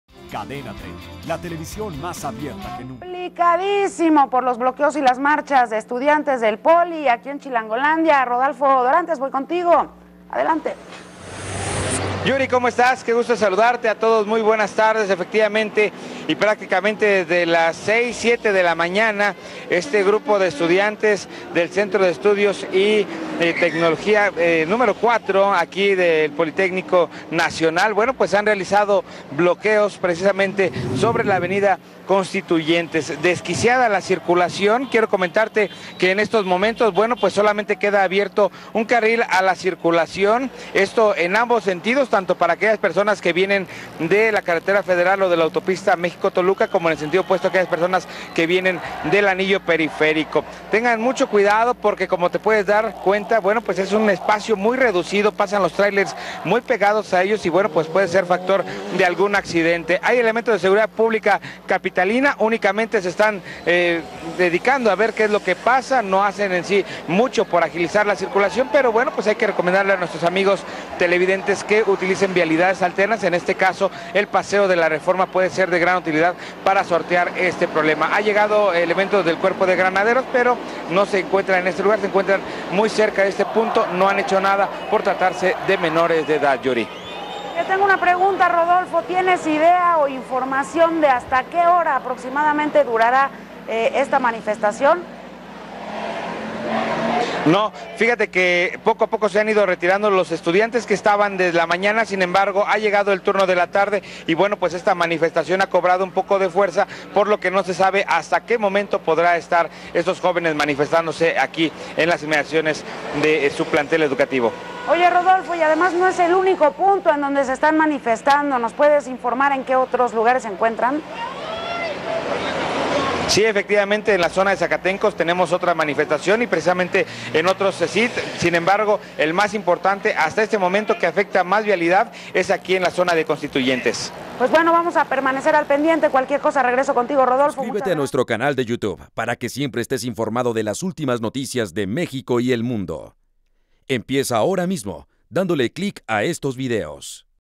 Estudiantes del IPN bloquean Avenida Constituyentes
26 septiembre 2014

Estudiantes del IPN número 4 bloquean Avenida Constituyentes. Estudiantes del IPN ocasionan caos vial en Constituyentes.